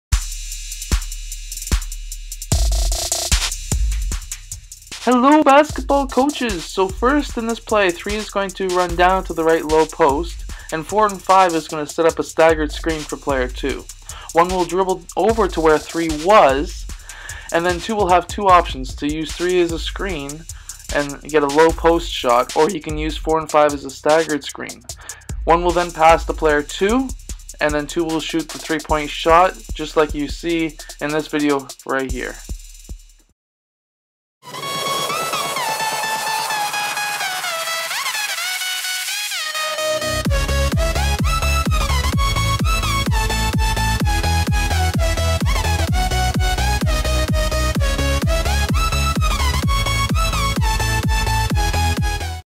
UCONN Huskies 1-4 Double Screen Away Offense | 1-4 Basketball Set Offense
UCONN 1-4 Double Screen Away Offense is a 1-4 Basketball play that starts with player 3 cutting to the opposite post and your point guard filling his place on the court. Now the wing on the far side of the court will be able to use player 3 in the post as a screen or he could use the two post players as a screen for a three point shot. This is a great 1-4 Basketball play to use, try the 1-4 Basketball Offense with your team and let us know how it works out.